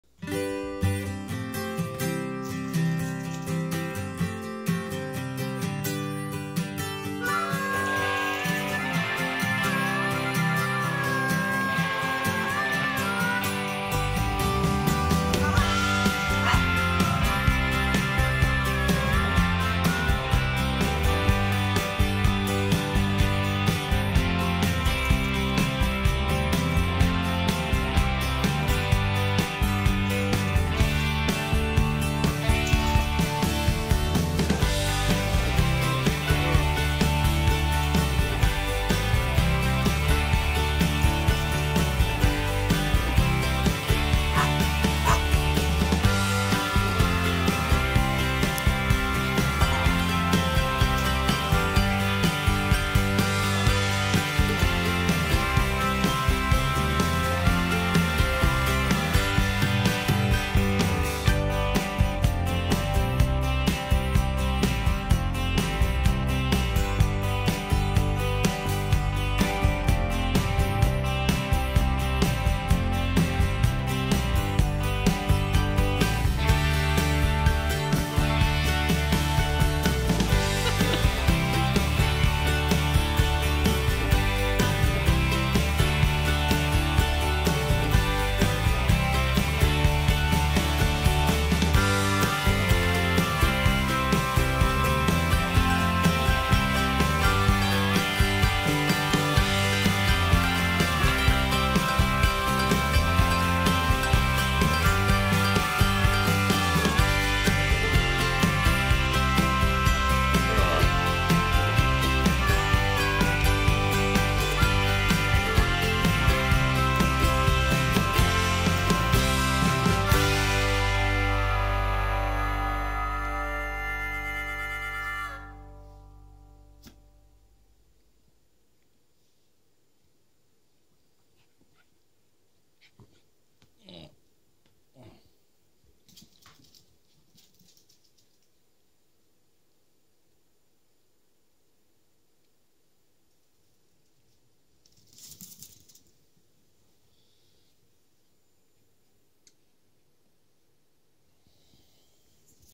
Scruffy helping Millie carry her toy while playing fetch
Scruffy being helpful and joining in as always.